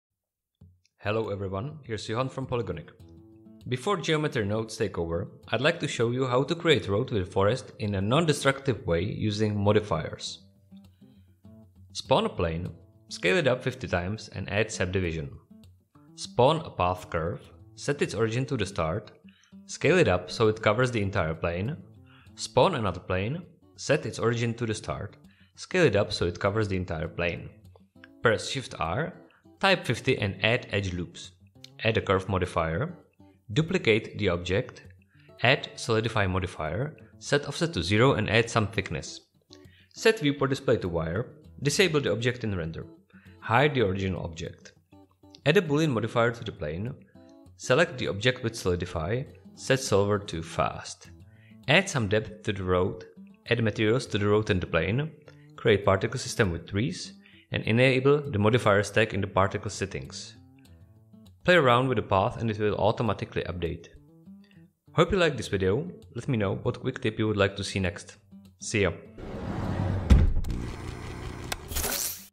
[botaniq] Non-destructive forest road
Johan shows how to make a forest road with a non-destructive approach. The botaniq scatter system reacts to the road and recalculates when changed.

If you are interested in more tips and tricks, go through our official documentation